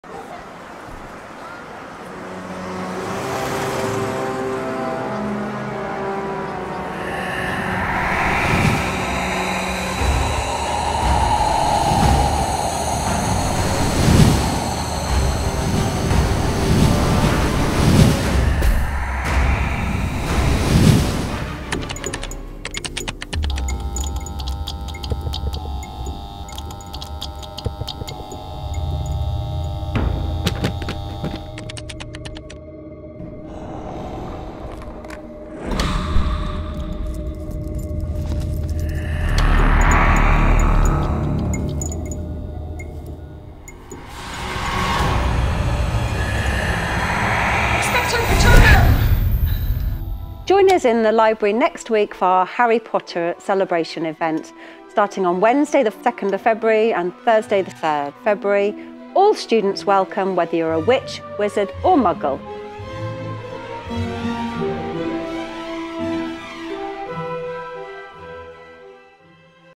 Harry Potter and The Great Library Event - TBH
Professor Bowles invites Tor Bridge High students to journey through the wonderful world of Harry Potter.